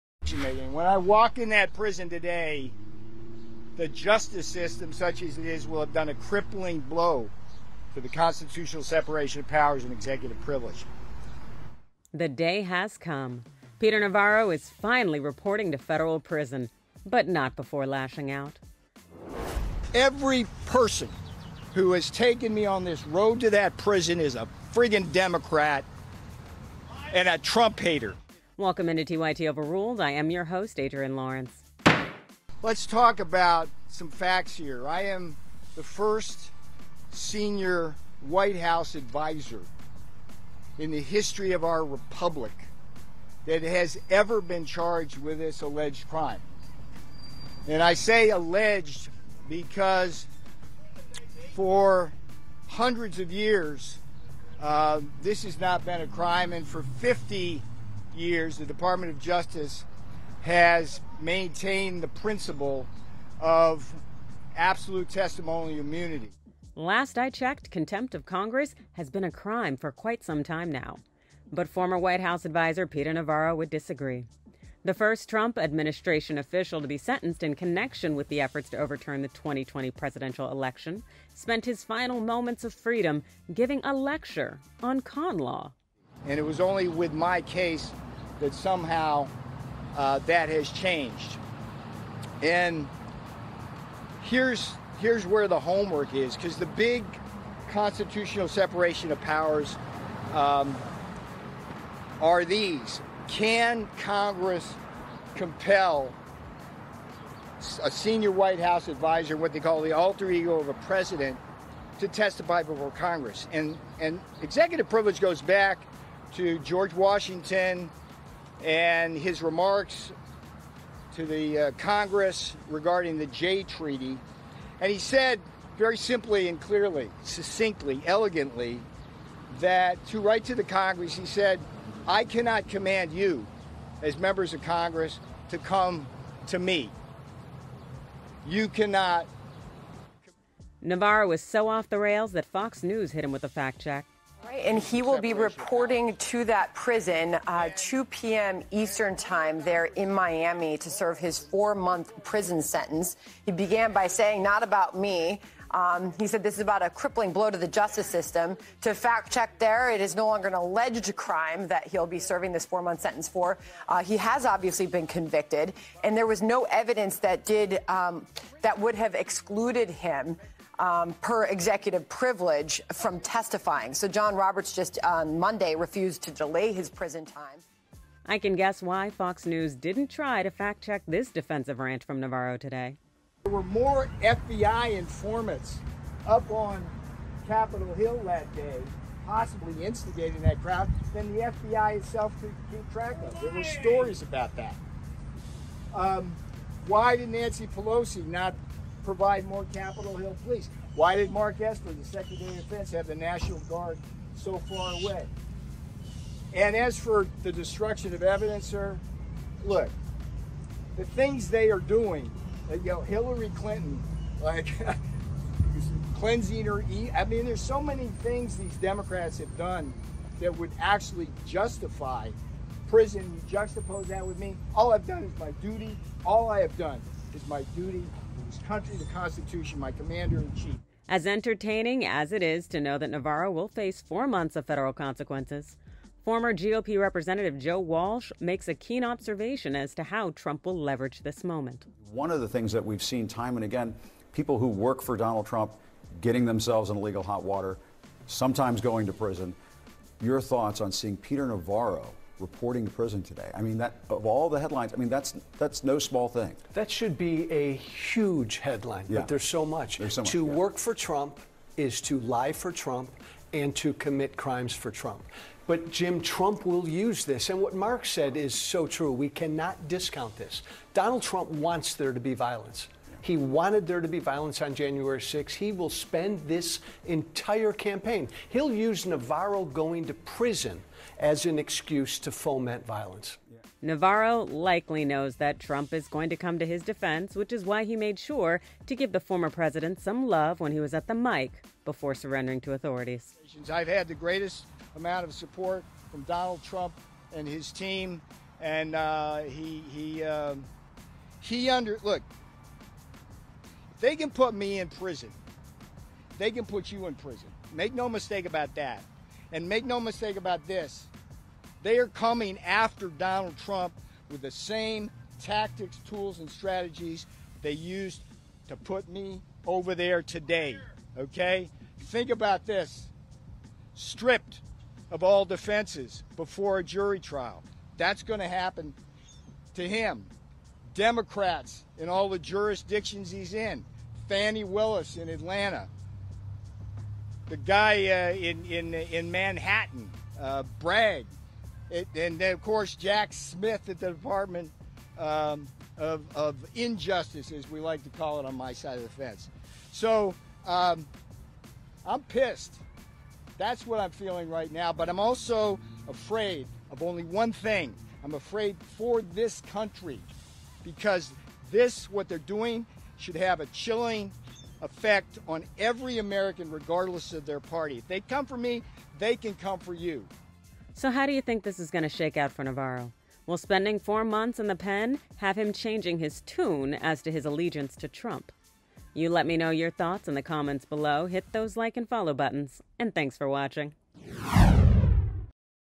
Peter Navarro FREAKS OUT Before Reporting To Prison
Former White House advisor Peter Navarro had some choice words today before he headed to jail for 4 months. Even Fox News had to fact-check him! Adrienne Lawrence breaks it down on Rebel HQ.